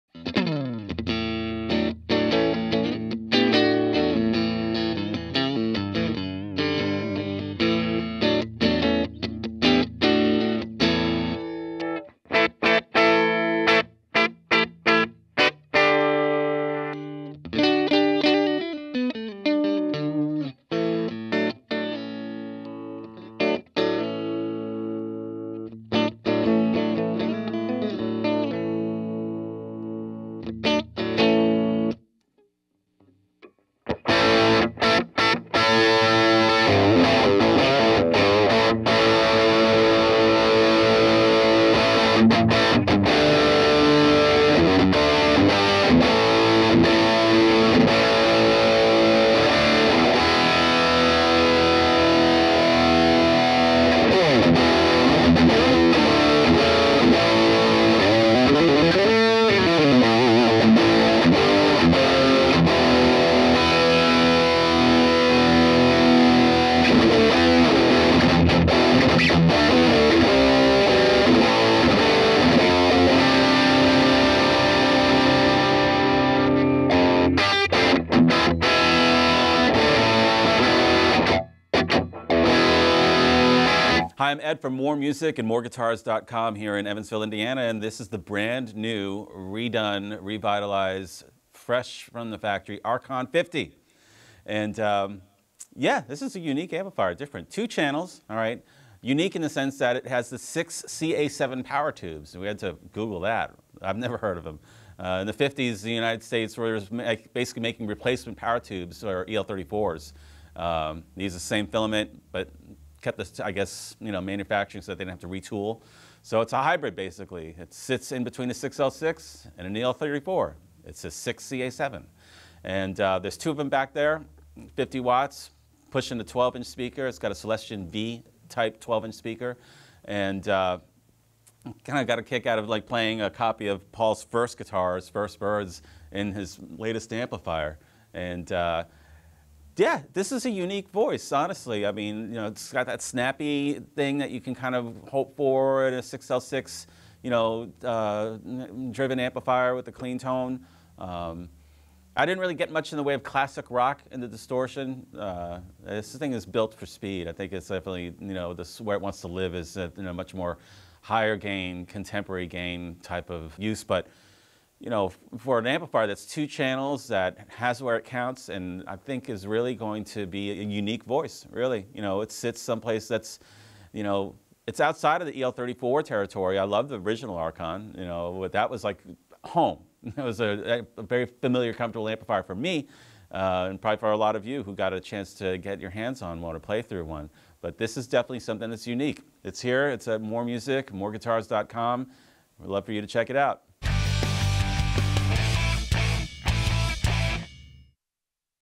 PRS Archon 50 Watt 1 X 12 Combo
The idea of a major redesign of an extremely successful amp platform would be immediately quashed by most manufacturers, but PRS's re-envisioning of the Archon not only increased the versatility of the amp - it decreased the price! This two-channel beast has been loved by metal players for its insane high-gain tones as well as its sparkling cleans. The Archon's new power section features two 6CA7 power tubes, which deliver smoother highs than 6L6's and higher headroom and tighter bass than EL34s. Five gain stages up front mean the snarling harmonic distortion is there when you need it, but rolled back you'll find the tones for any sort of rock. PRS shoots, and scores!